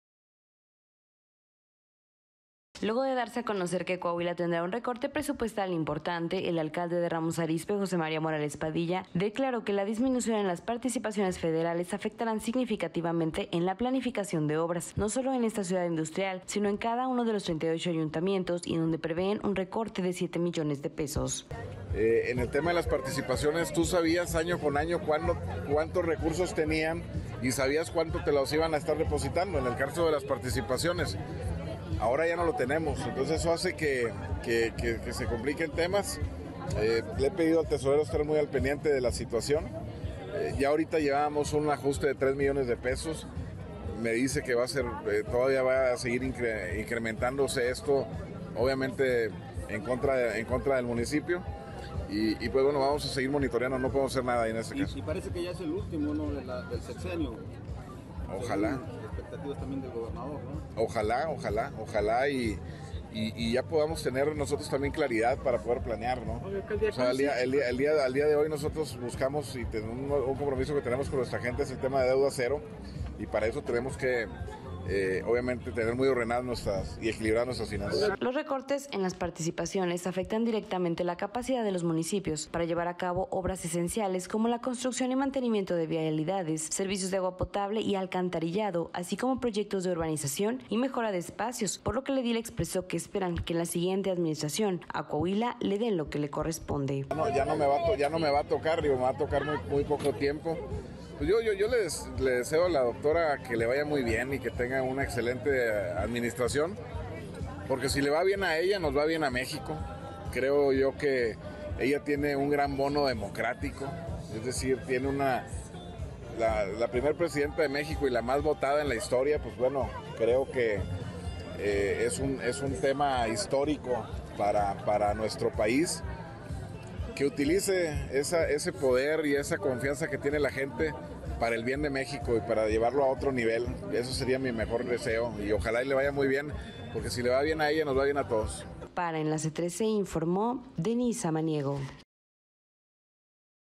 Preocupan recortes presupuestales en Coahuila en el impacto de planeaciones de obra
Coahuila | El alcalde de RamosArizpe, José María Morales Padilla, alertó sobre el impacto del recorte presupuestal en las obras municipales, afectando a los 38 ayuntamientos de Coahuila.